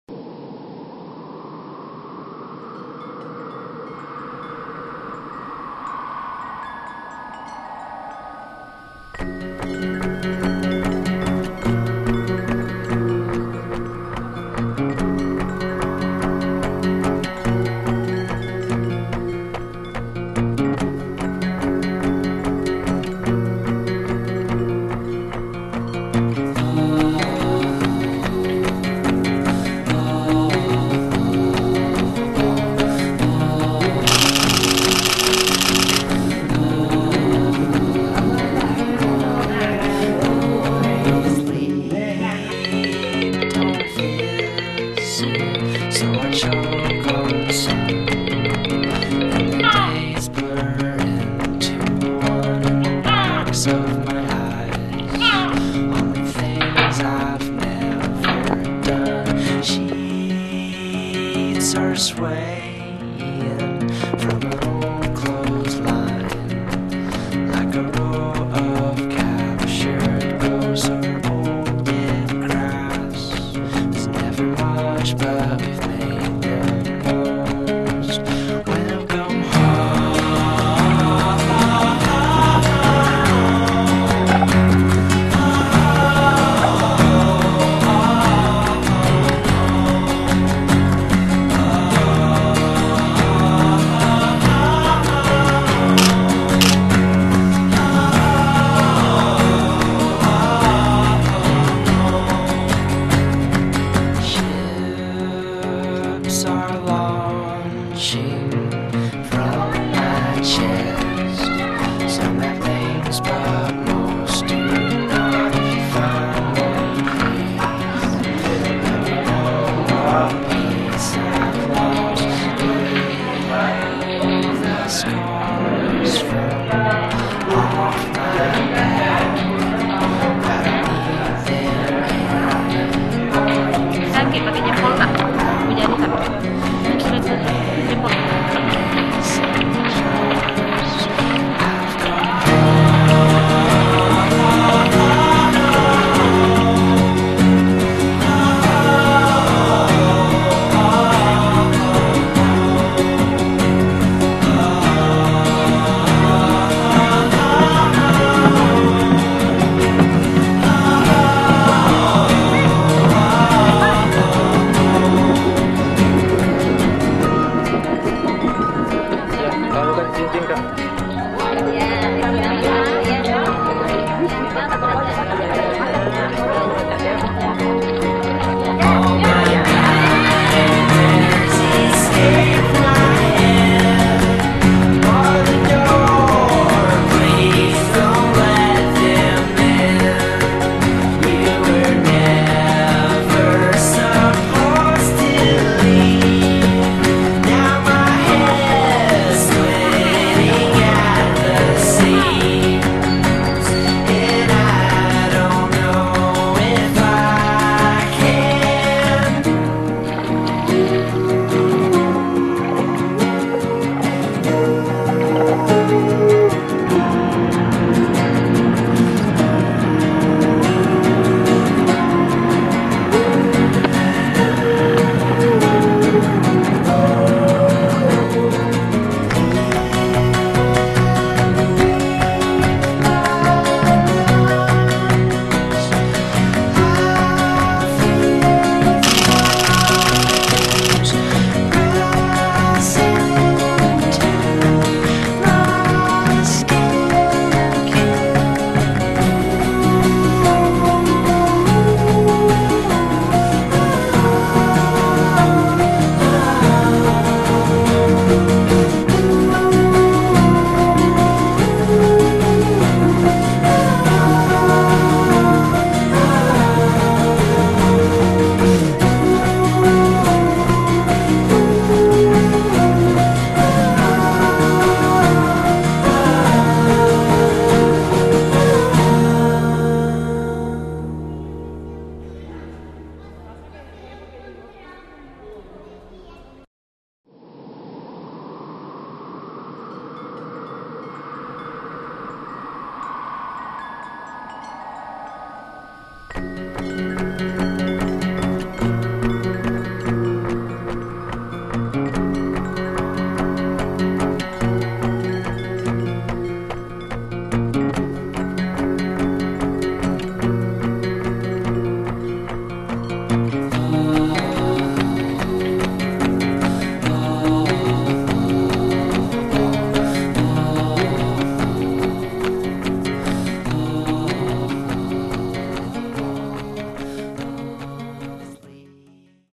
I AM NIKON MANDAR
FIlm pendek ini adalah dokumenter tentang kebudayaan Mandar. Pembuatannya terinspirasi dari iklan Nikon, "I AM NIKON".